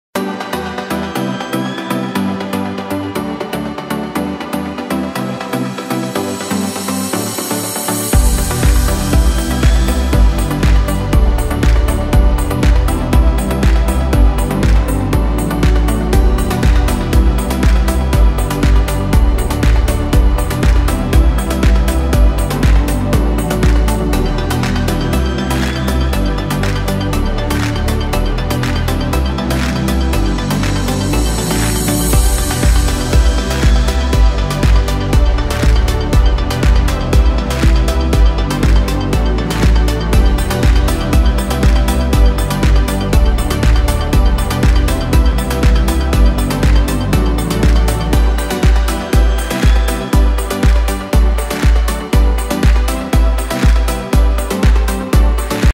HORNO DE CONVECCIÓN, MAXICONV, PARA NEGOCIOS, PANADERÍAS, PIZZERÍAS Y RESTAURANTES EN EL SALVADOR
Horno con preciso control de temperatura, para una cocción uniforme. Para más información entra a
¿tienes algún requerimiento en equipos o restaurantes?